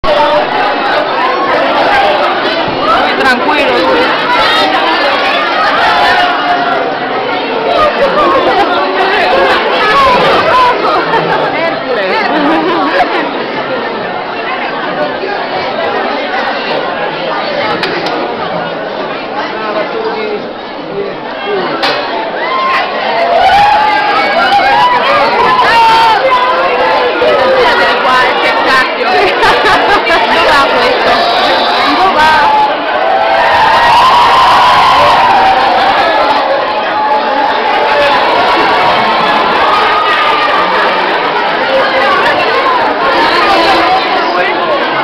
Misha Collins & Kevin Sorbo - JIB Convention - 3 aprile 2010
Misha incontra Kevin Sorbo fuori dalla sala foto, poi Kevin passa e va verso la sala dei panel